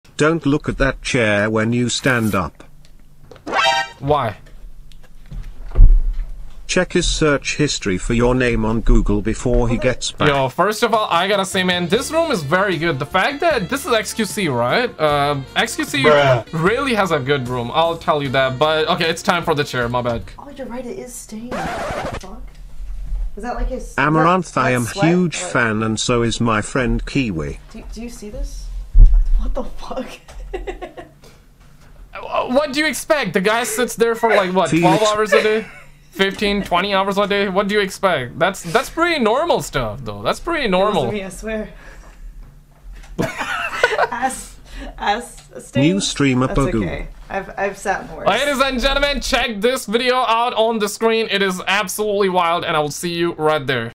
Amouranth Just Did This on xQC Stream... 😬
Amouranth laughs at xQc's chair stains